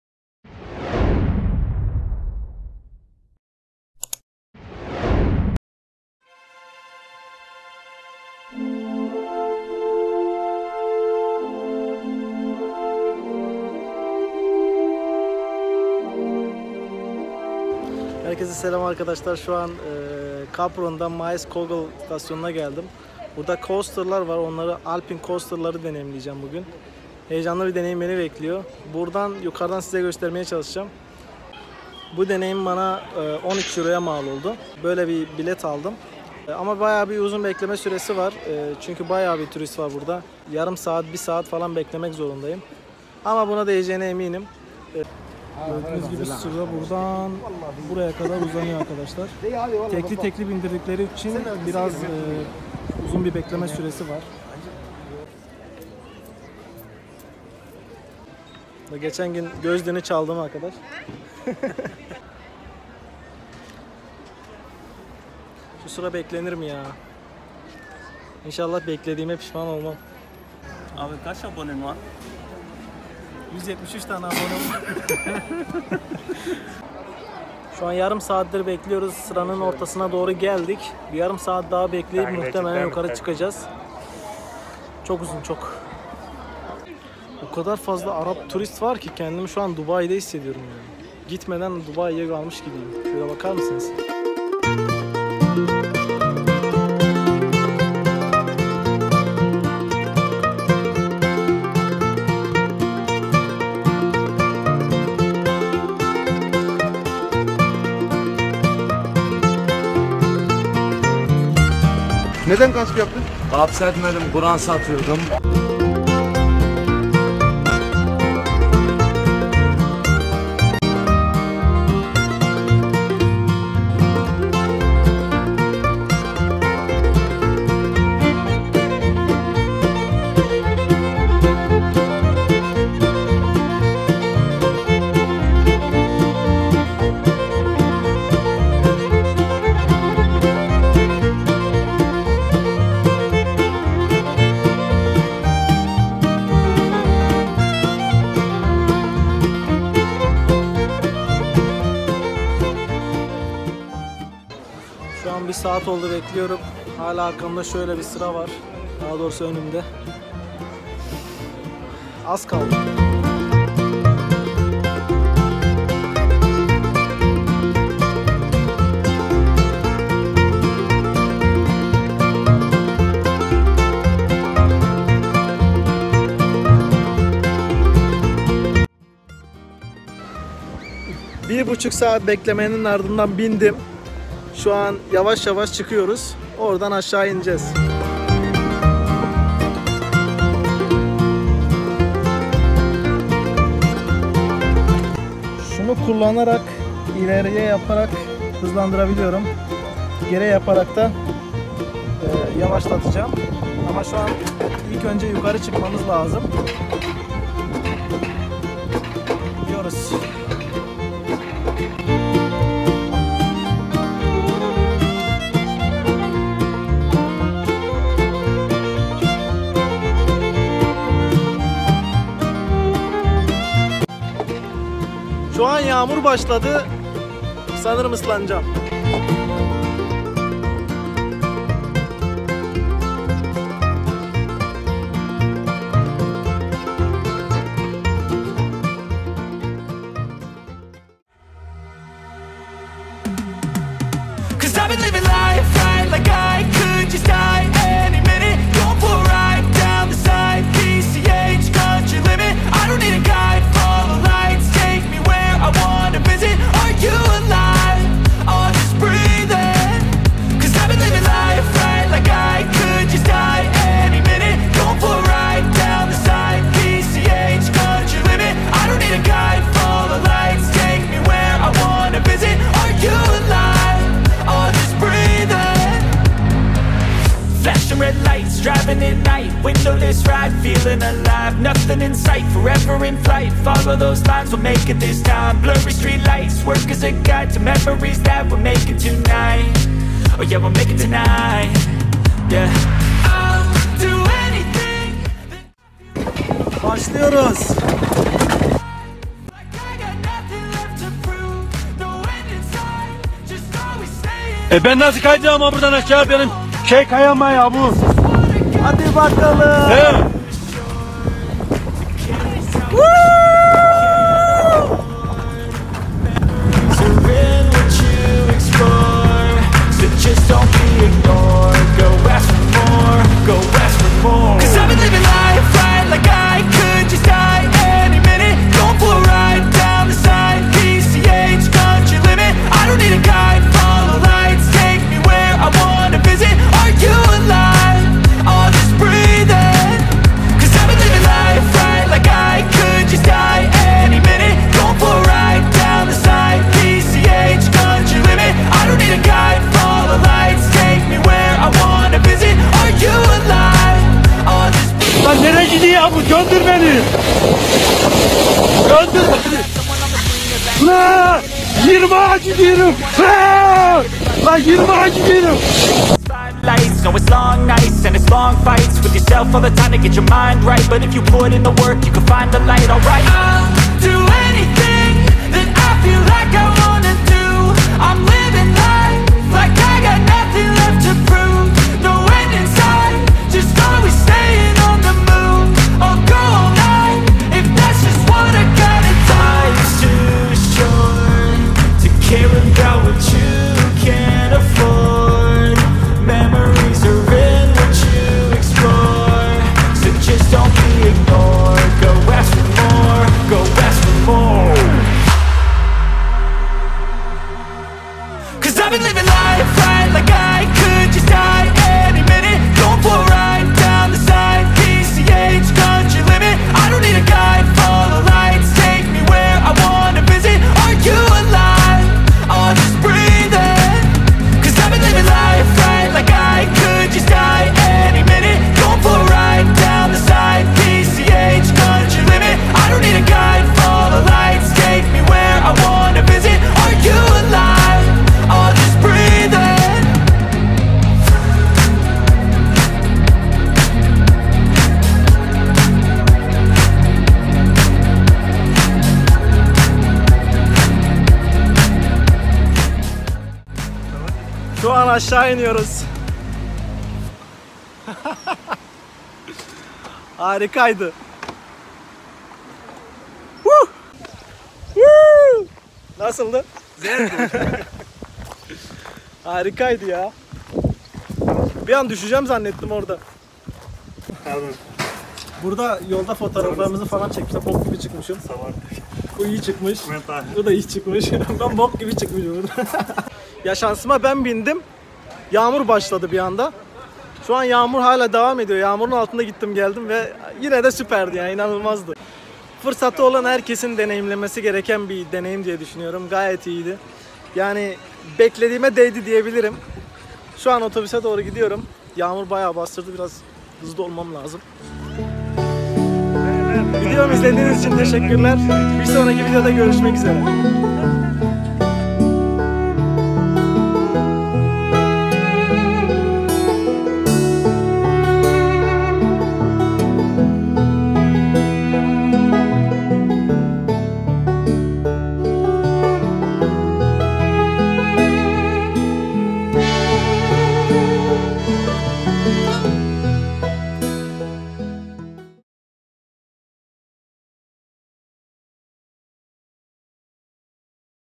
Yağmurda Coaster Deneyimi - Avusturya🇦🇹
Selam ben Mehmet Amiroğlu. Bugün benim için süper bir deneyim olan Alpine Coaster ile karşınızdayım. Maiskogel, Kaprun’da bulunan bu tesiste heyecan verici anları sizinle paylaşmak istedim. umarım beğenirsiniz, iyi seyirler.☺️